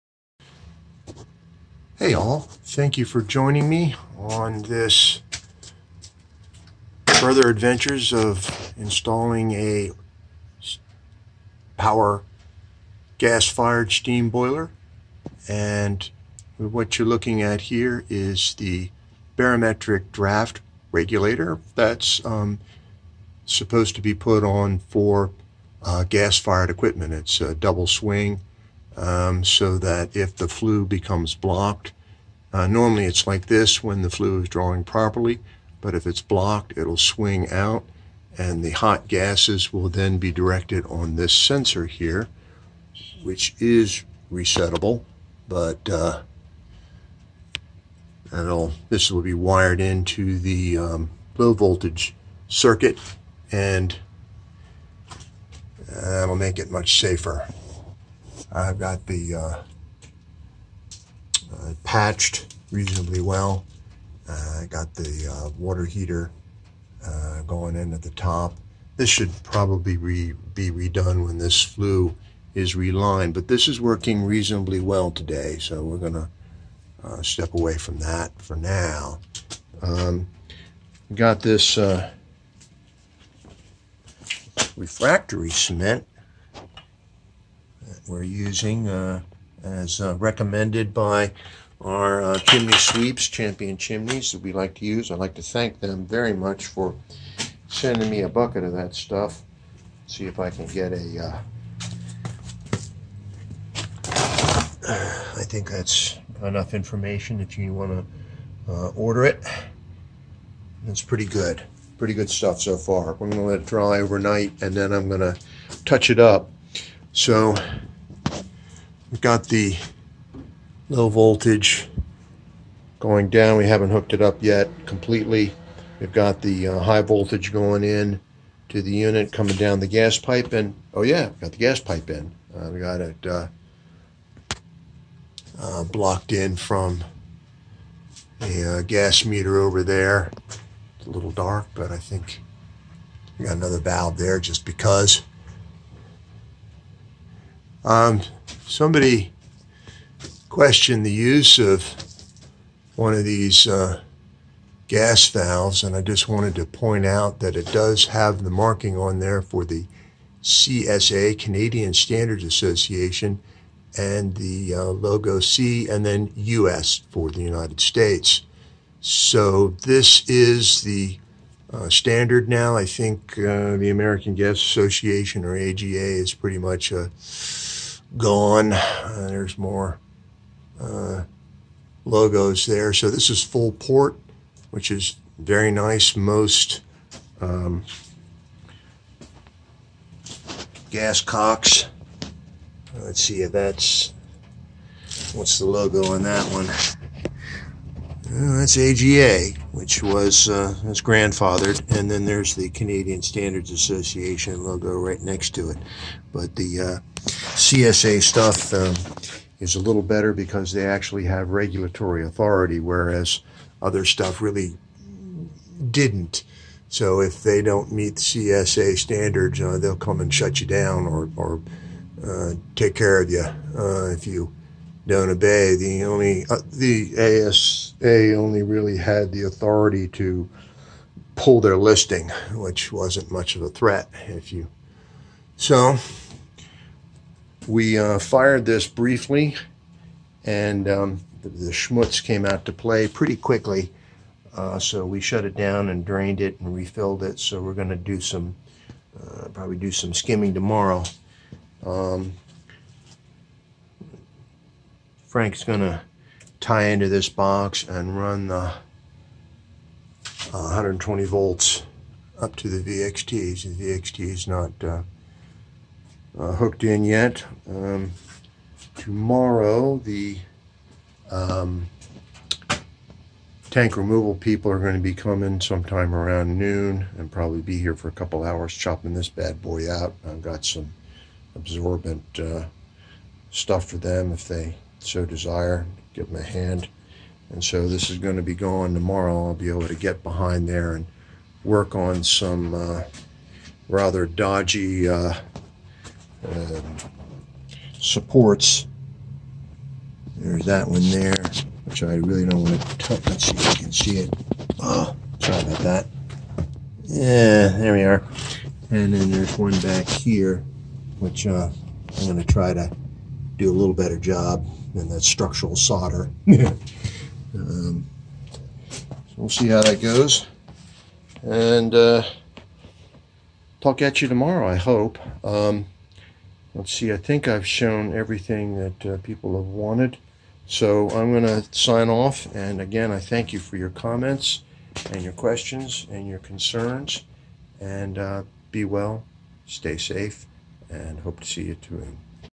Almost Finished Install Of Gas Fired Steam Boiler.
Weil Mclain SGO-4 Steam Boiler with Midco Gas power burner is almost done.
Hydrolevel VXT-120 needs to be hooked up, the burner tuned, and the initial scum removed from the boiler insides